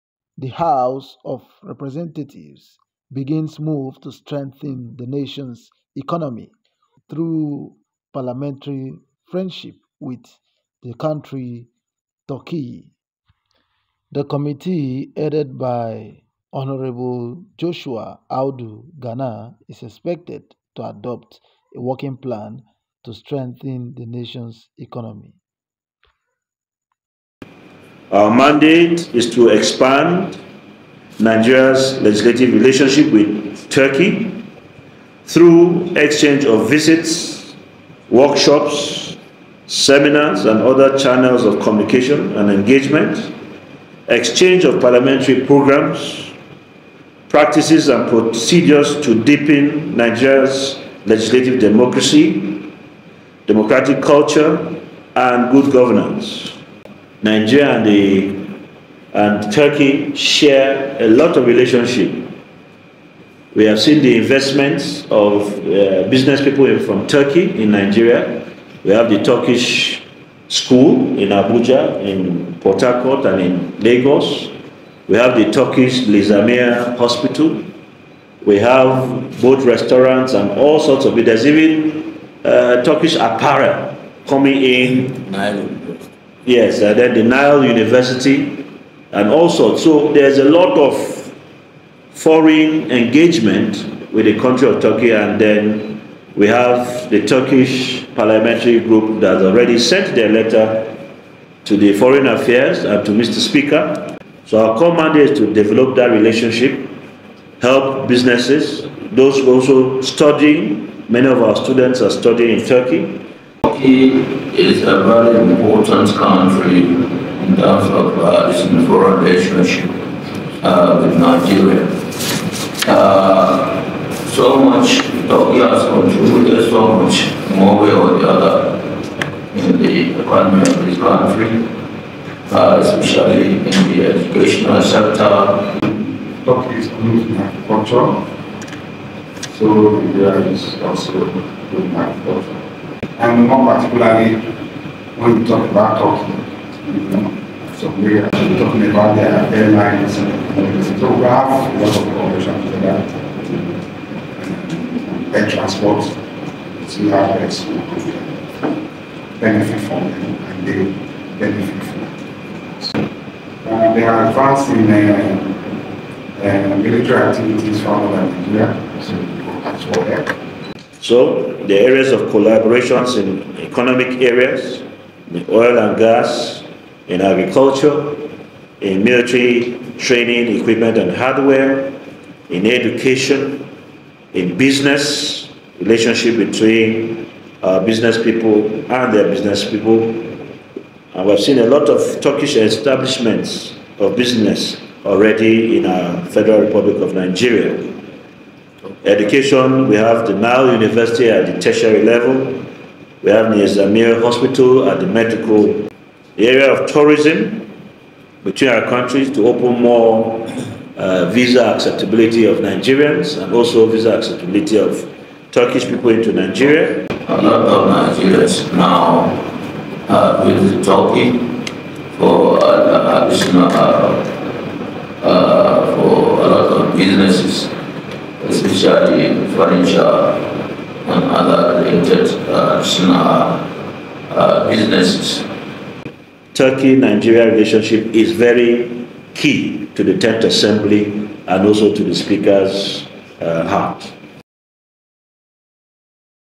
Hon Joshua Audu Gana
Hon. Joshua Gana seeks stronger bond with Turkiye to strengthen and  improve massive development across Nigeria...